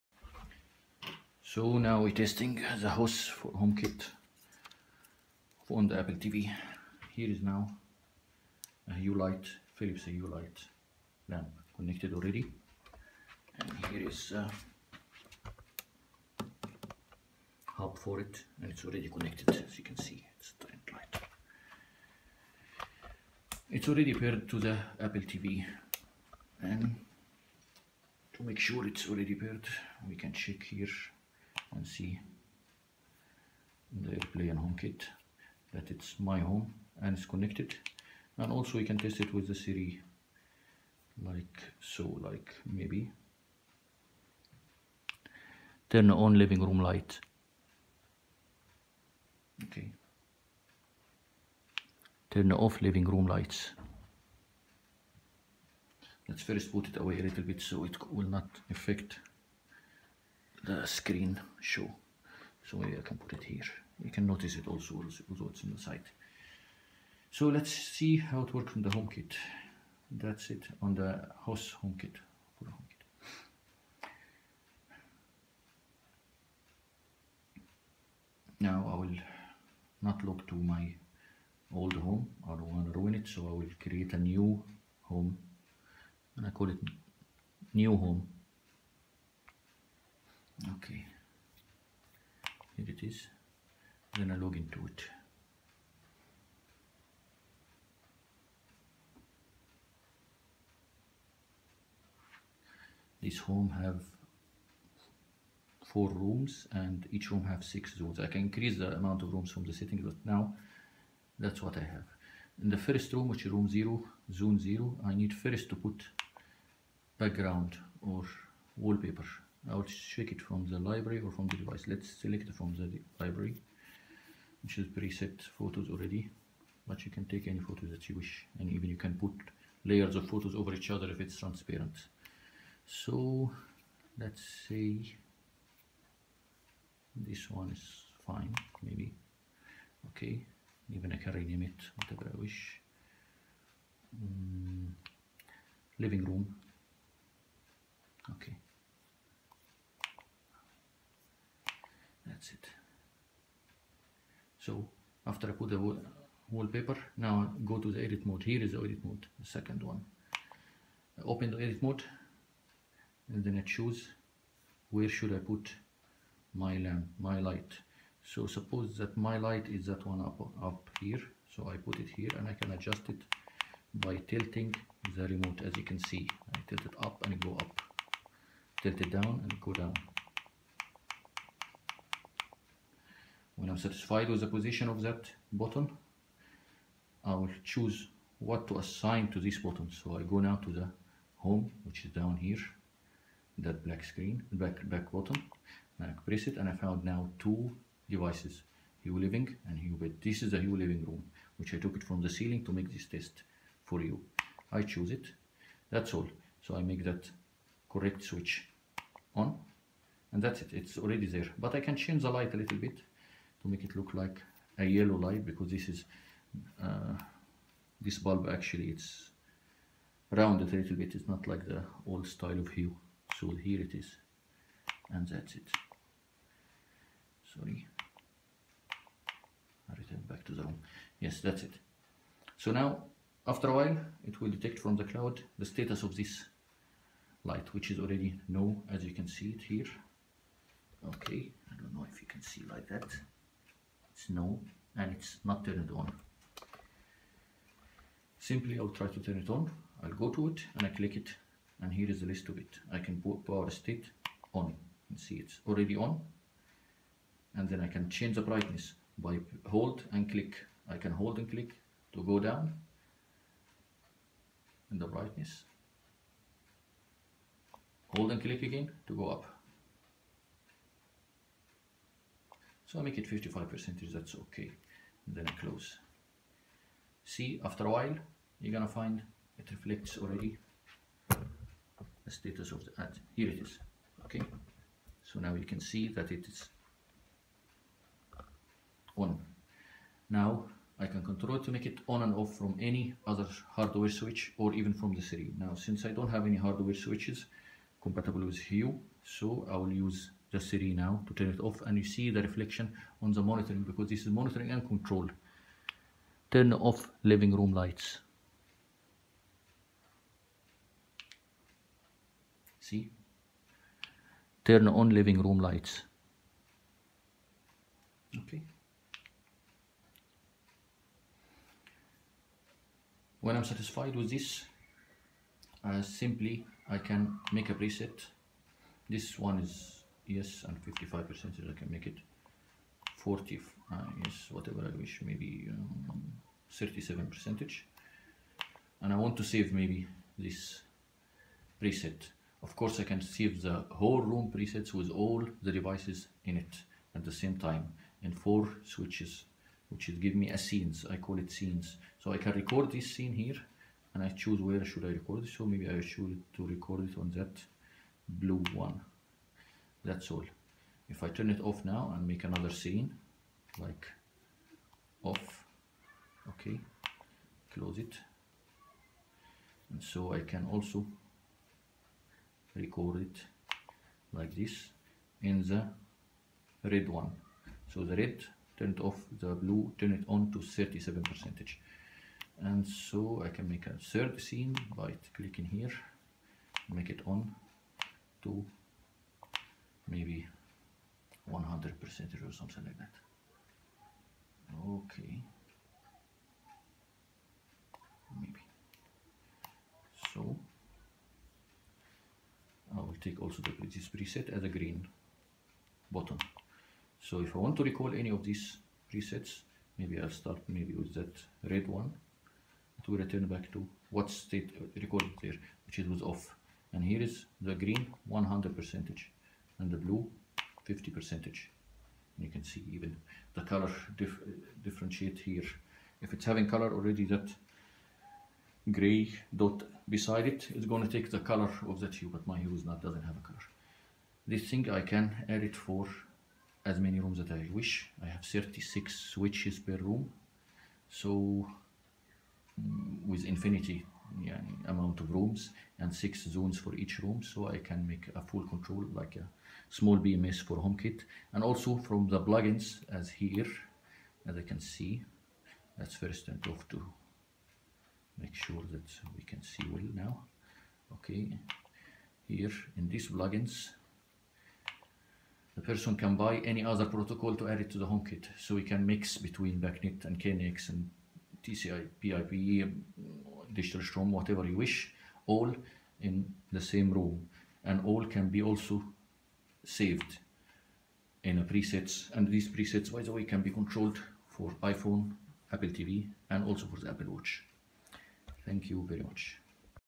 HOS Smart home For HomeKit Live Real Test for AppleTV
Real Testing HOS Smart Home With Philips Hue Light bulb as a HomeKit Device,
Experience a dynamic Smart Home App represents each device in a 3D Live animation visually detected using Ai technology, acts and sounds exactly as it do in real world.